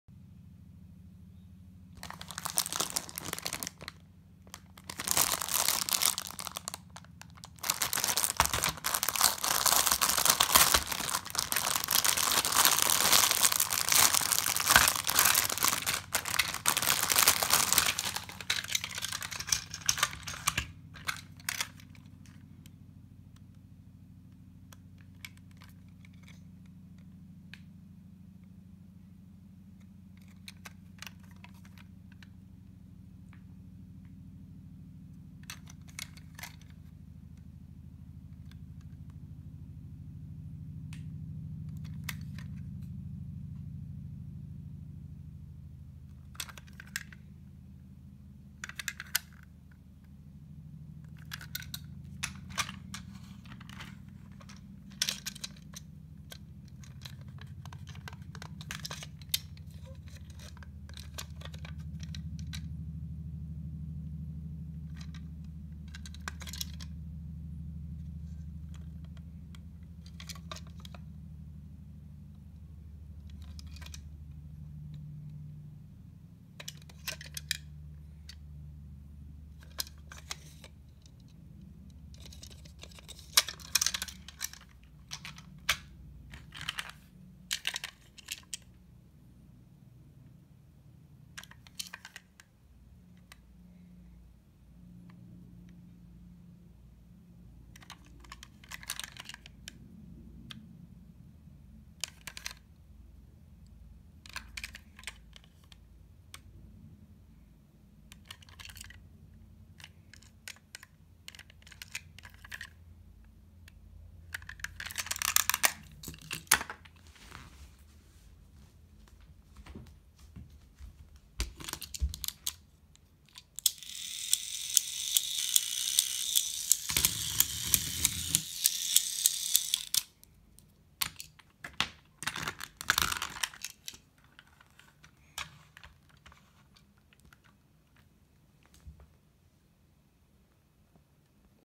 (貴重)中古プラレール リゾートしらかみ 開封
1～2年前に購入。ギヤ割れ、中間車のシミ以外見たら状態はいいです。